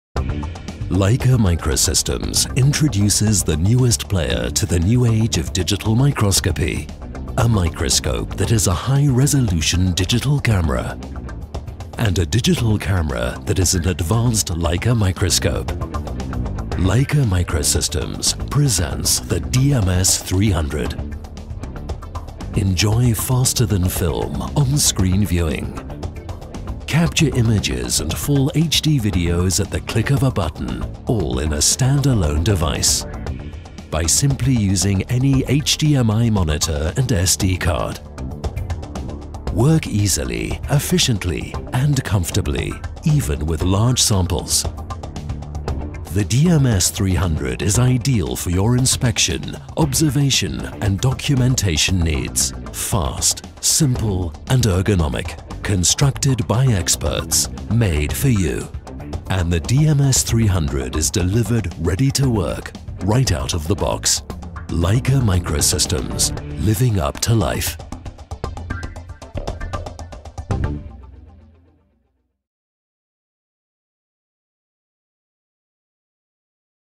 Microscopio Digitale Leica DMS 300
Leica DMS300 un nuovo protagonista tra i microscopi digitali Leica Microsystems.
 Dietro al design esteticamente gradevole si nasconde un sistema con un’ottica e una fotocamera di ottime prestazioni.
 In modo stand alone con monitor HD o con computer, grazie a una tecnologia digitale superiore